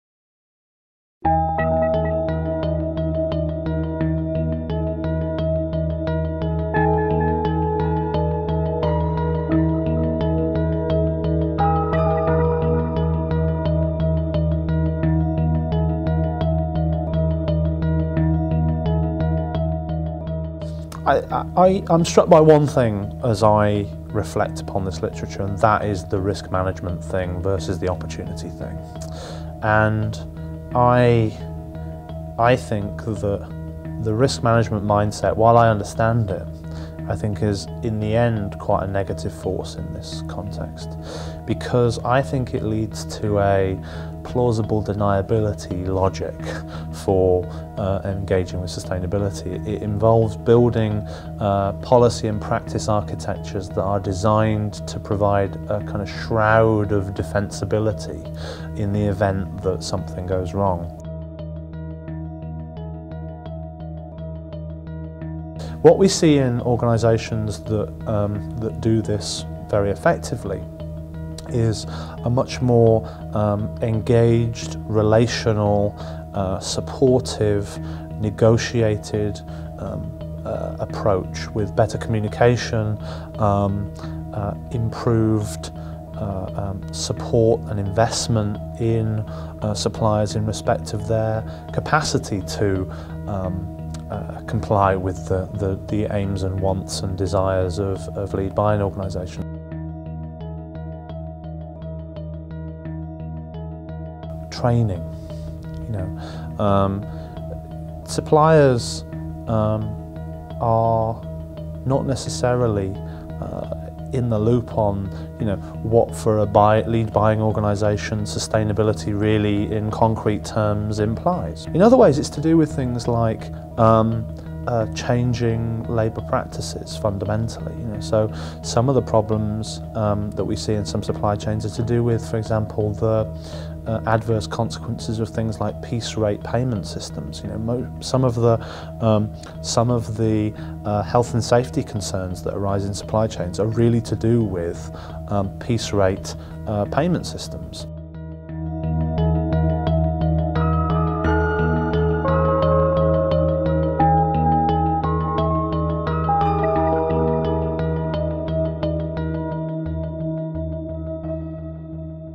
How to Manage Sustainable Global Supply Chains
Professor Stephen Brammer talks about his recent research: Managing Sustainable Global Supply Chains: A Systematic Review of the Body of Knowledge